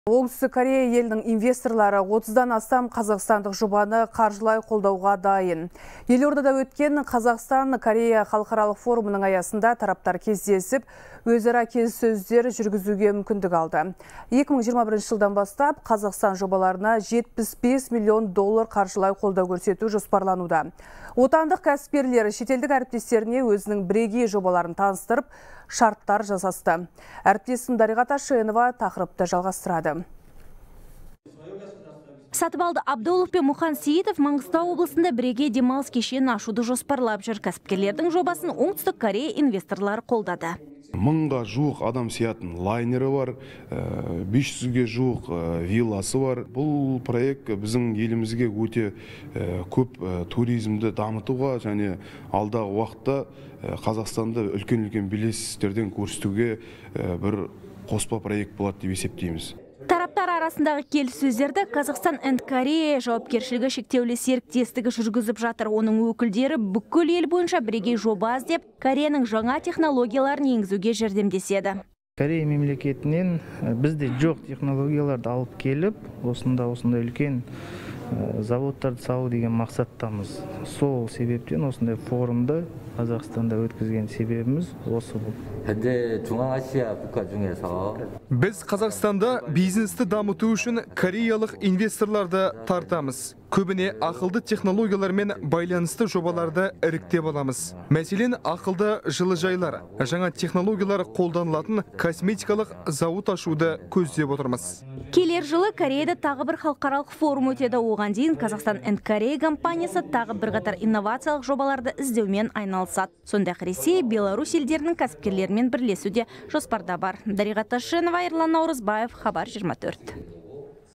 Кореялық инвесторлар 30-дан астам қазақстандық жобаны қолдауға дайын
Оңтүстік Корея елінің инвесторлары 30-дан астам қазақстандық жобаны қаржылай қолдауға дайын. Елордада өткен Қазақстан Корея халықаралық форумының аясында тараптар кездесіп, өзара келіссөздер жүргізуге мүмкіндік алды. 2021 жылдан бастап Қазақстан жобаларына 75 миллион доллар каржылай колдау корсету жоспарлануда. Отандық кәсіпкерлер шетелдік әріптестеріне өзінің бірегей жобаларын таныстырып, шарттар жасасты.